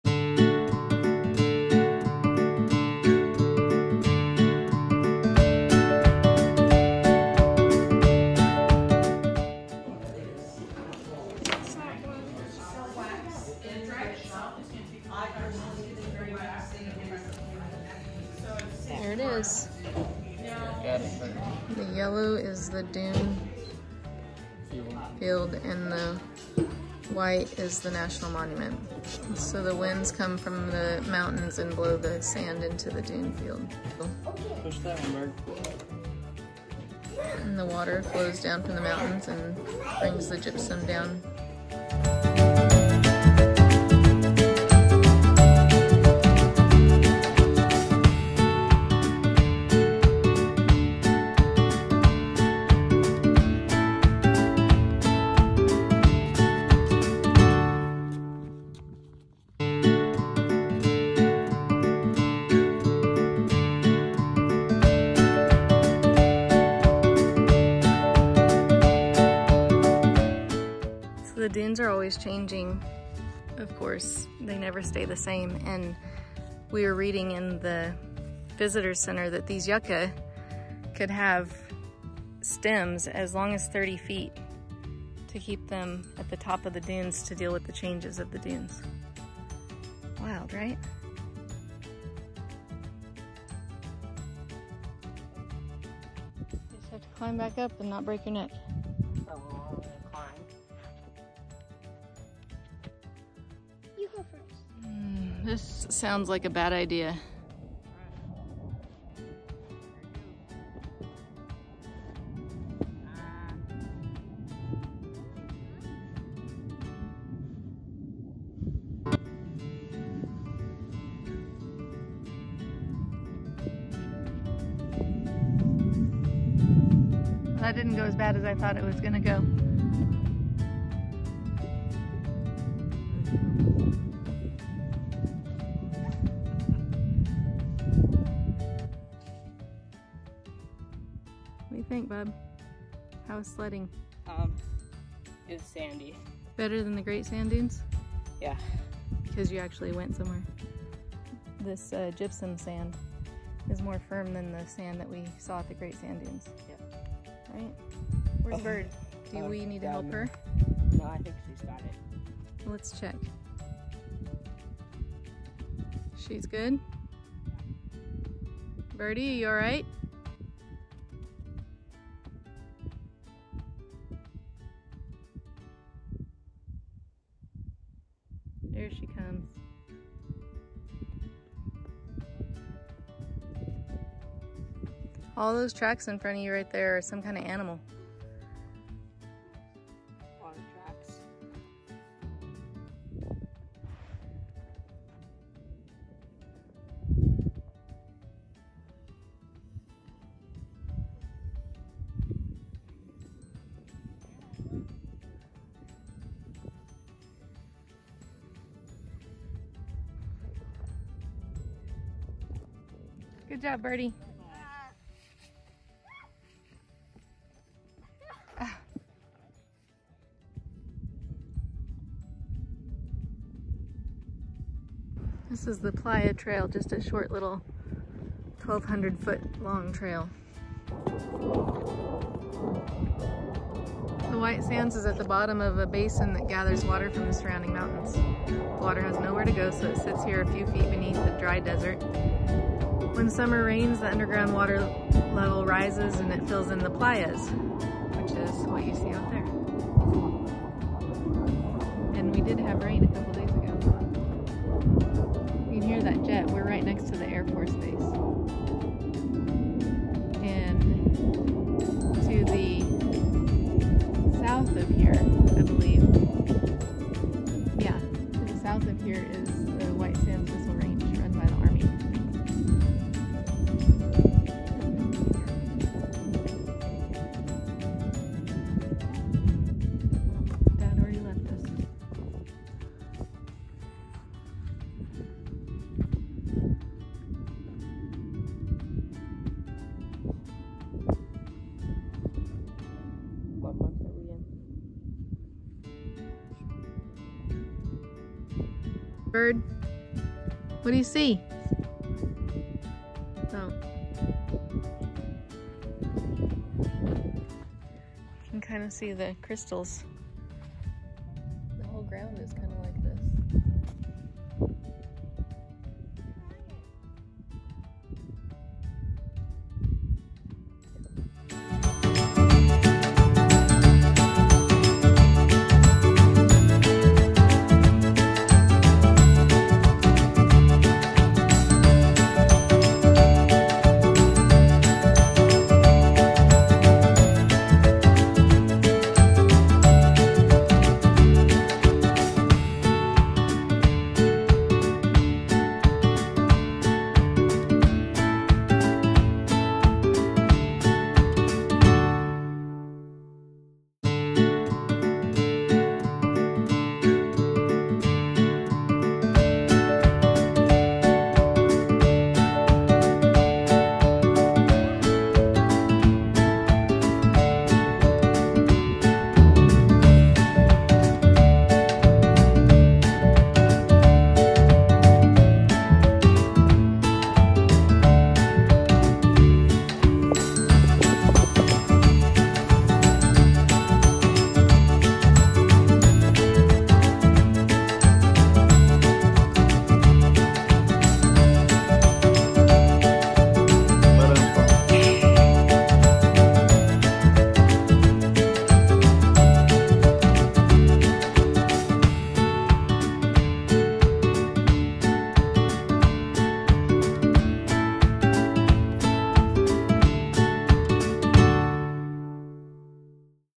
White Sands, NM
A beautiful day exploring White Sands, New Mexico with the fam. Marked another national park off our list and did a little hiking and sledding in the gypsum dunes.